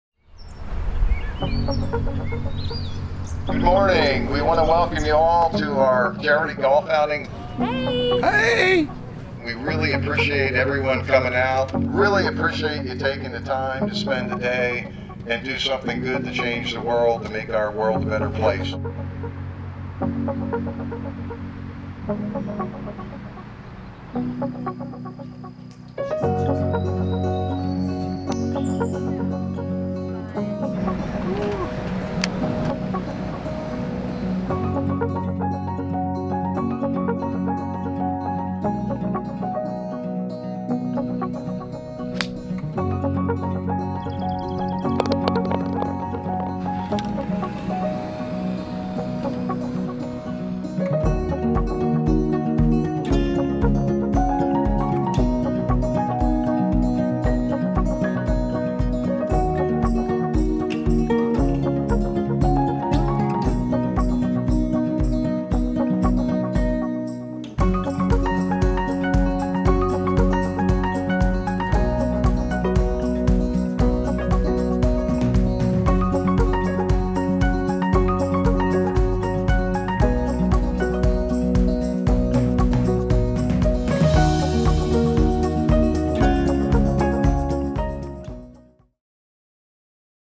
25th Annual Members 1st Charity Golf Classic
Golfers traveled from all over to participate in our 25th Annual Charity Golf Classic.  We are appreciative of all those who helped make this possible and supported our four beneficiaries.

A special shutout to our amazing sponsors and congrats again to The Salvation Army Harrisburg, Helping Harvest, Aaron’s Acres, and the Central PA Food Bank.